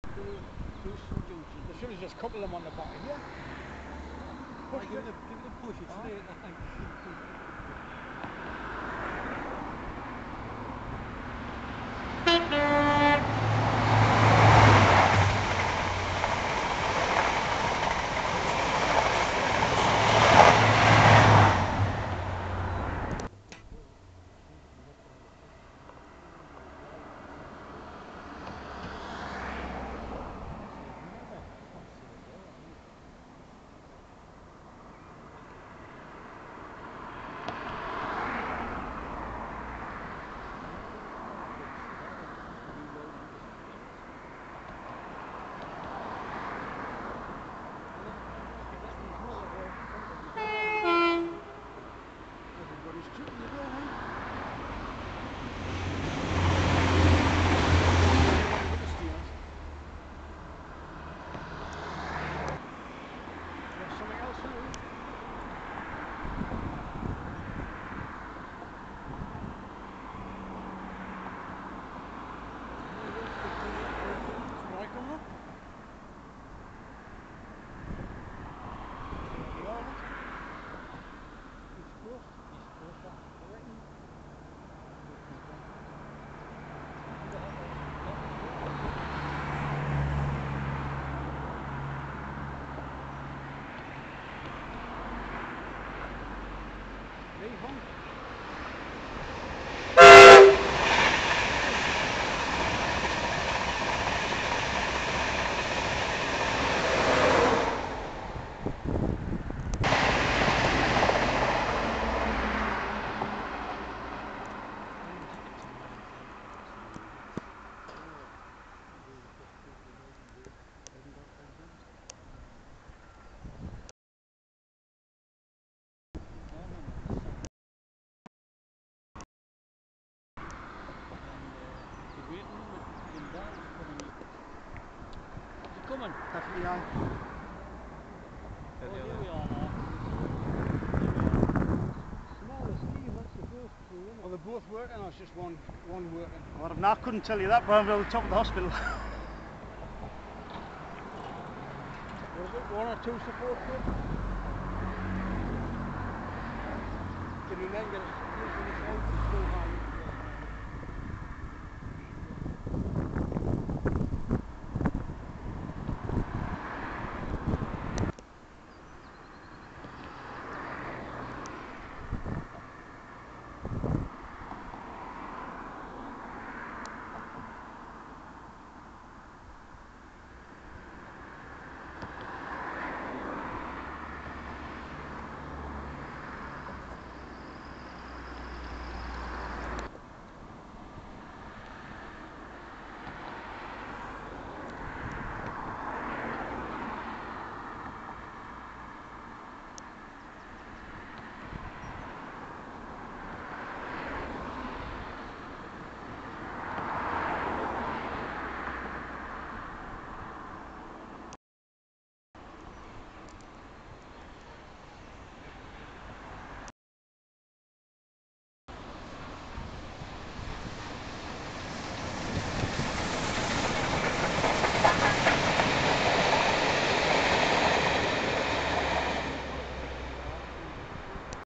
45407 and 44871 light engine 12/4/10
45407 and 44871 light engine from bo'ness to grosmont seen here at low fell in gateshead 12/4/10